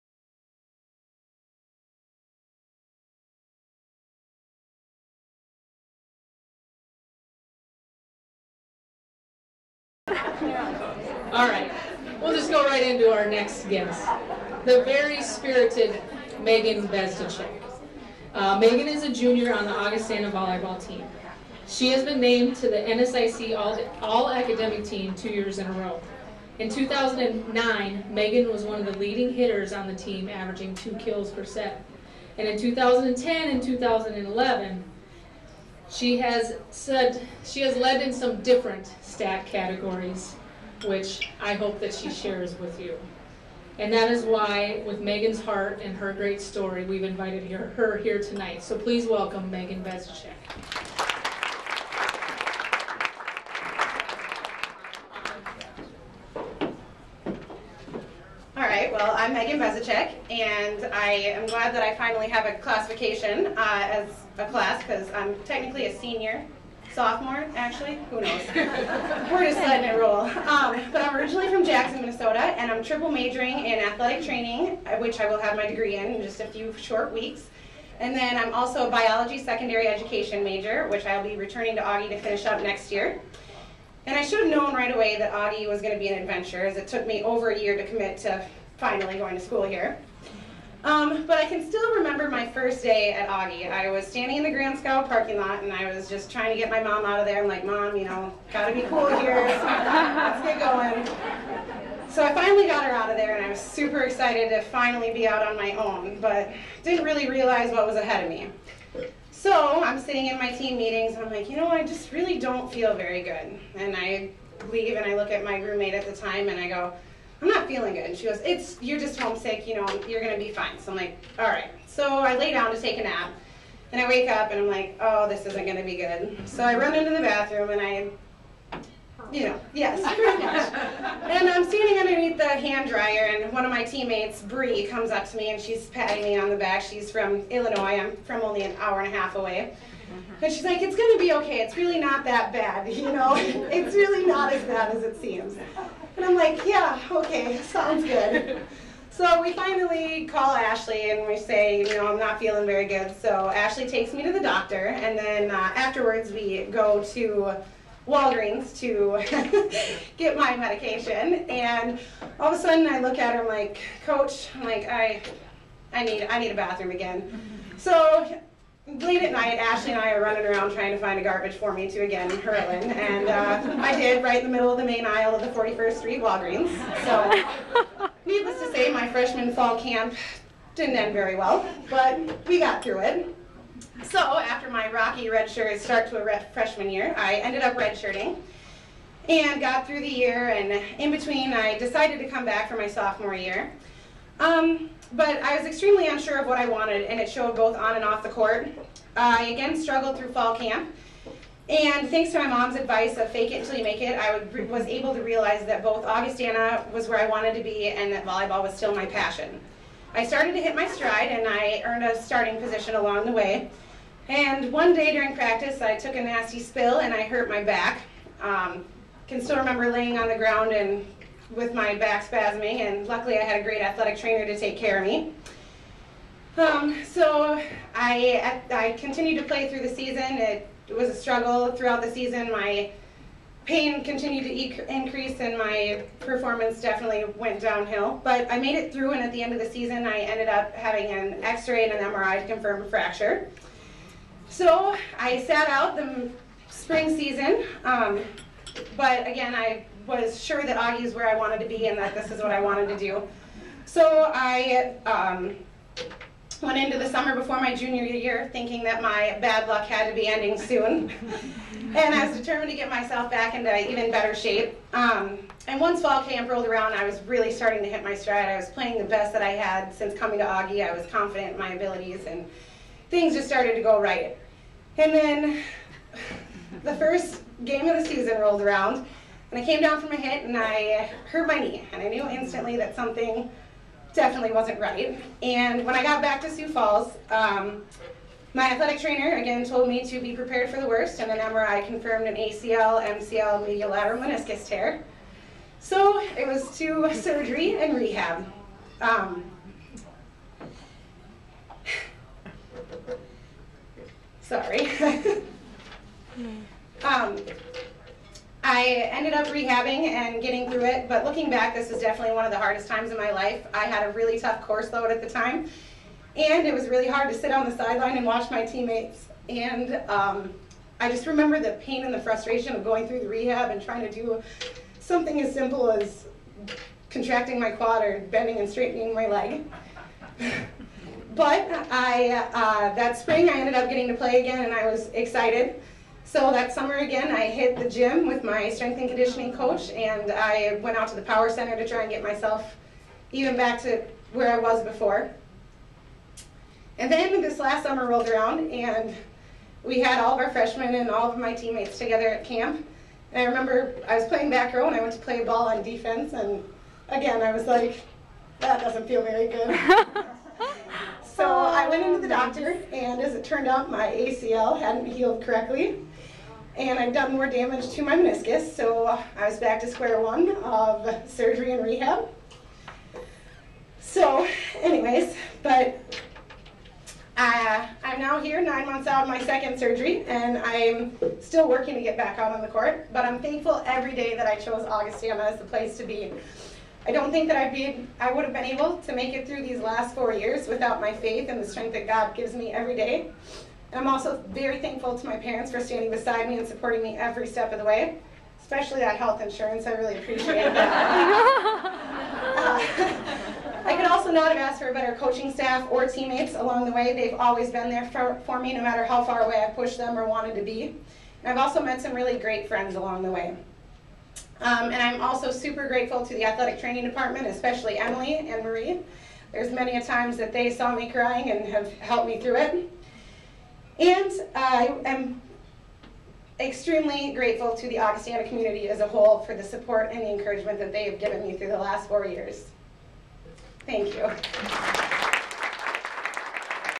Spring Soiree - Megan Bezdicek
Augustana student-athlete Megan Bezdicek speaking at the Augustana Spring Soirée held at the Minnehaha Country Club in Sioux Falls, SD on Thursday, April 26.